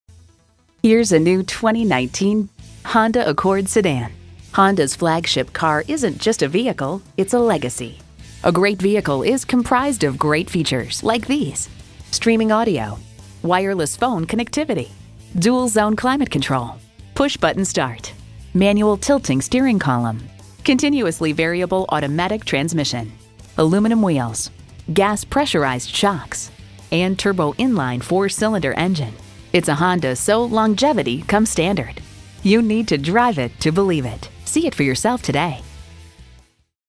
New 2019 Honda Accord Elizabeth City, NC #419223 - SOLD
Year: 2019
Make: Honda
Model: Accord
Trim: LX
Engine: 1.5T I4 DOHC 16V Turbocharged VTEC
Transmission: CVT
Color: Blue
Interior: Gray
Stock #: 419223
VIN: 1HGCV1F15KA052095

Address:
105 Tanglewood Parkway South